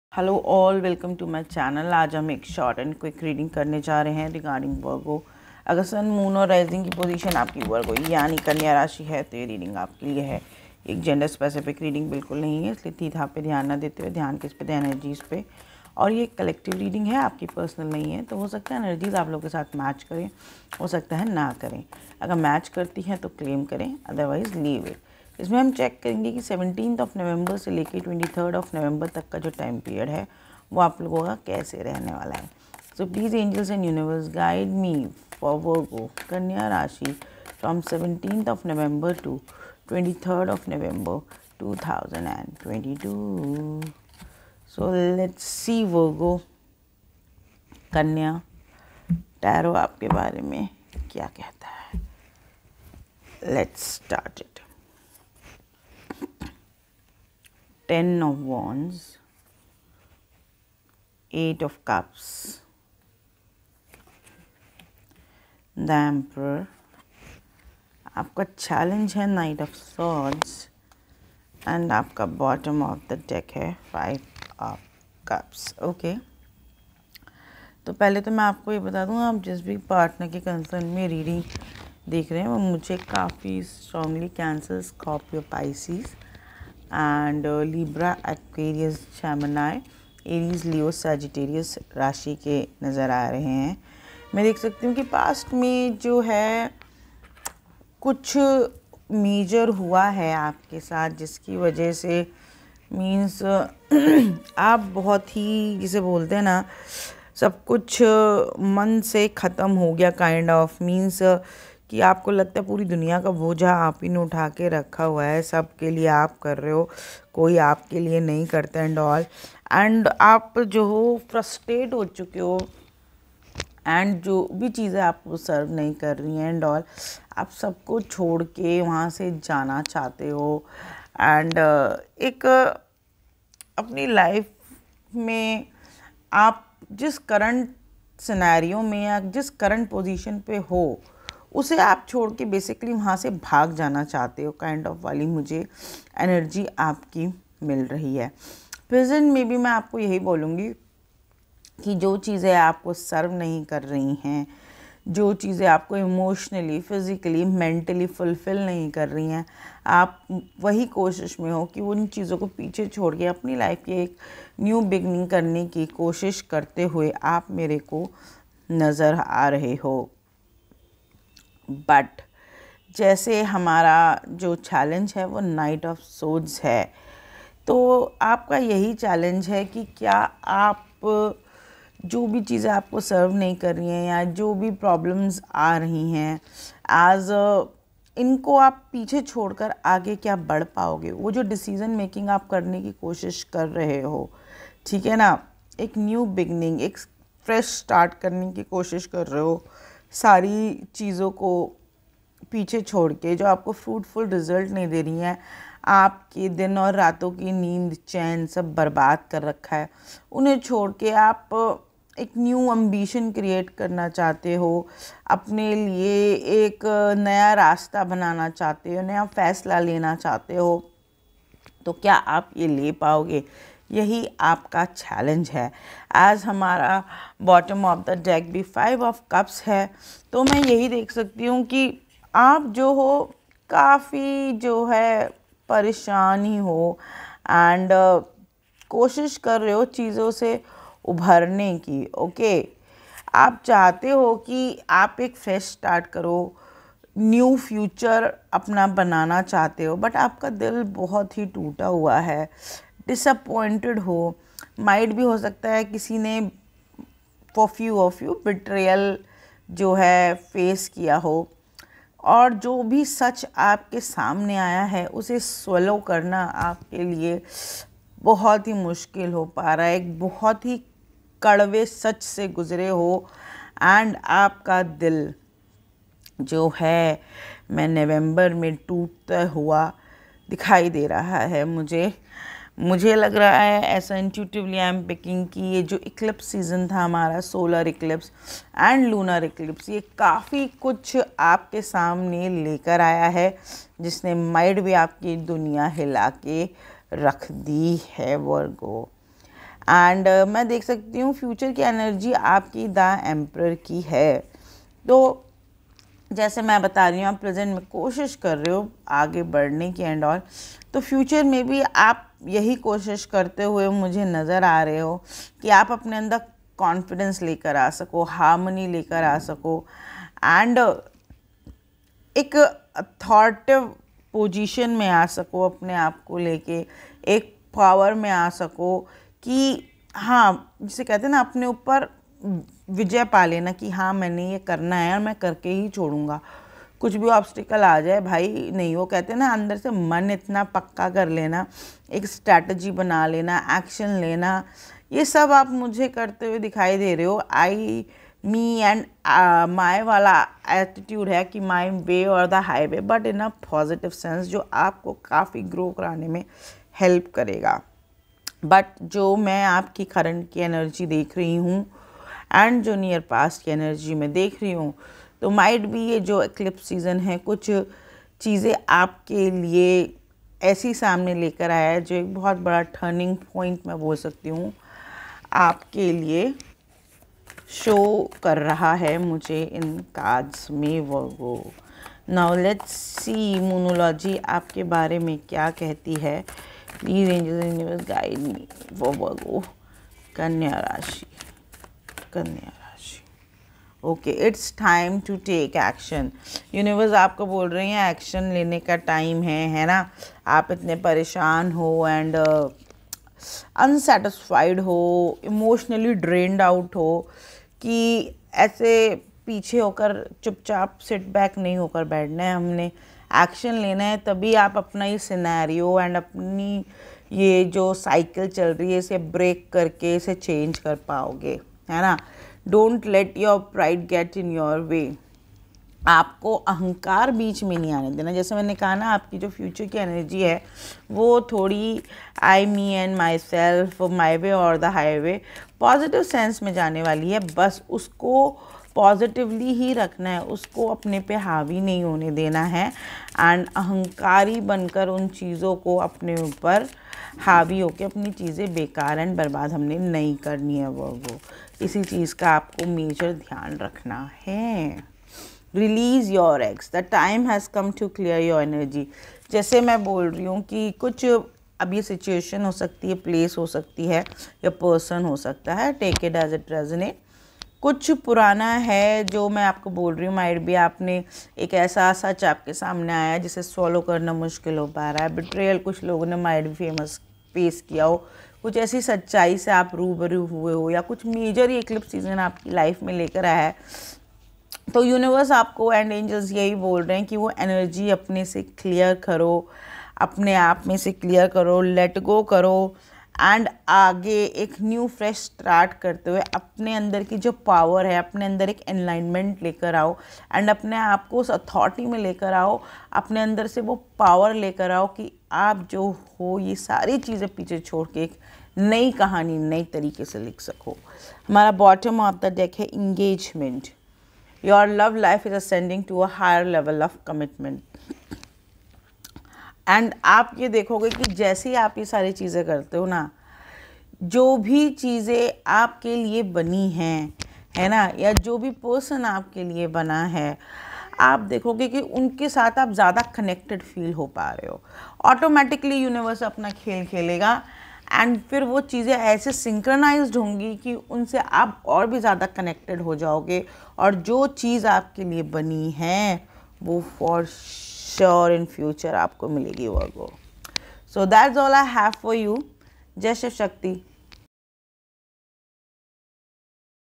💗 VIRGO ♍️ - Trying to move forward! 17th-23rd November 💗
Tarot Reading of virgo (kanya rashi) from 17th-23rd november 2022